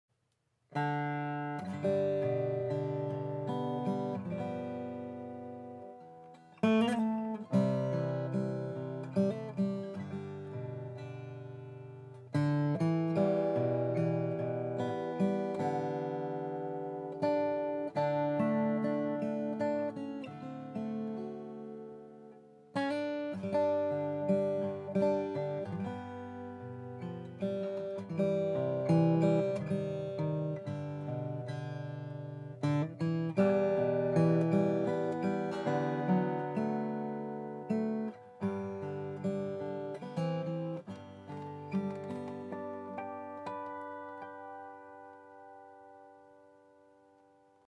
Vines SX Guitar at Guitar Gallery
or GUITAR GALLERY. Played by Lance Allen. Unbelievably sweet guitar!